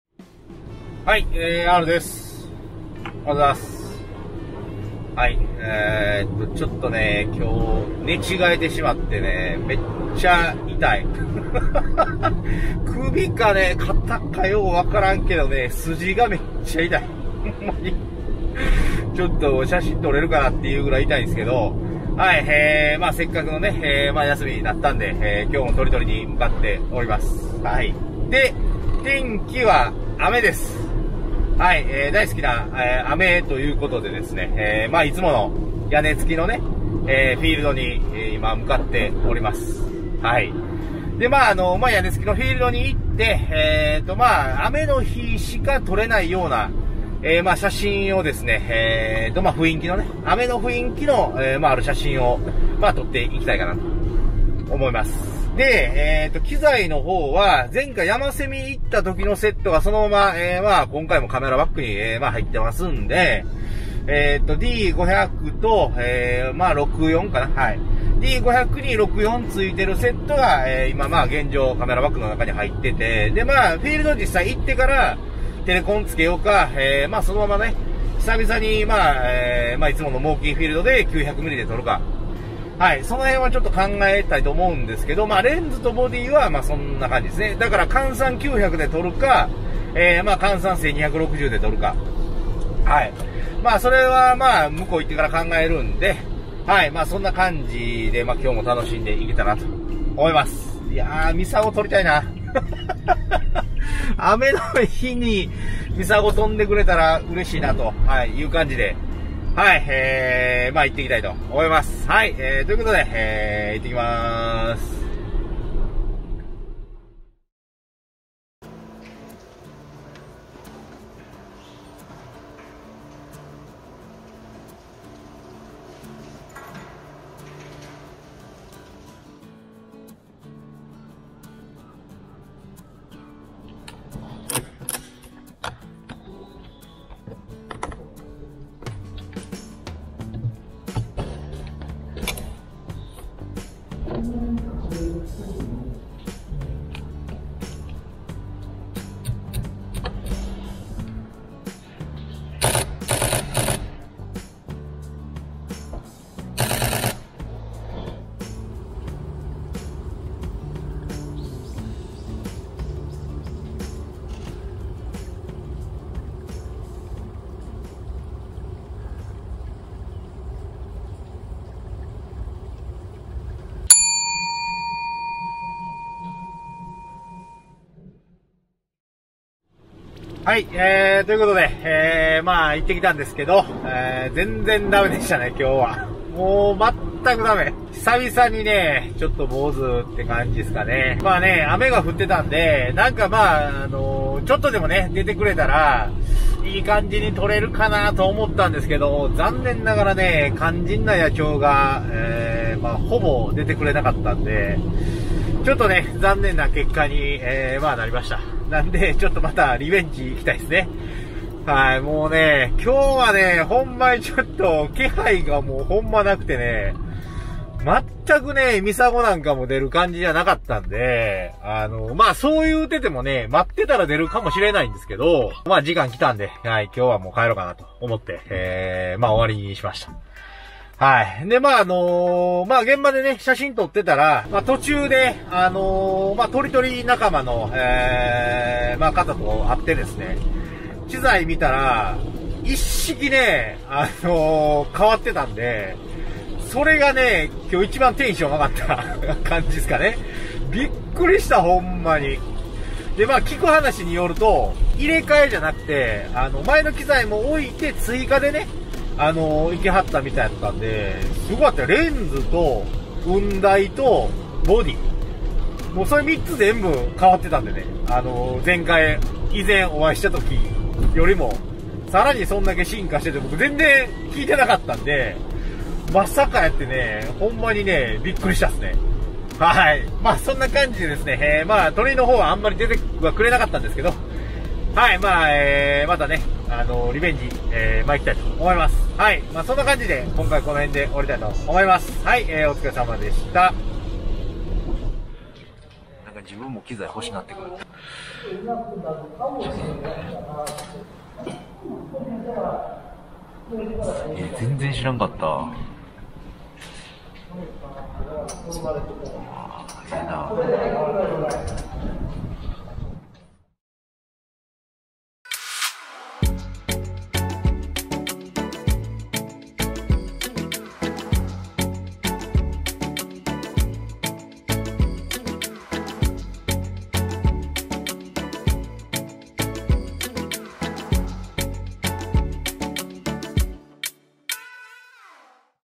【野鳥撮影】楽しみにしていた雨天撮影！のはずが…。- D500 × AF-S NIKKOR 600mm f/4E FL ED VR -
＊写真の再生時、画質設定はできれば4Kモードにてご視聴ください。

【使用機材】
D500

AF-S NIKKOR 600mm f/4E FL ED VR

【SNS】
LINE 友達追加